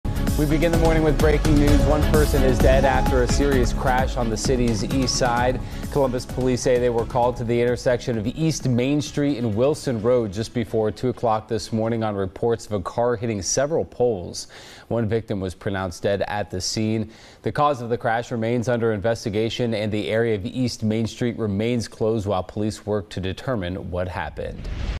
One dead after single-car crash on Columbus' east side
One person is dead after a single-car crash on Columbus' east side.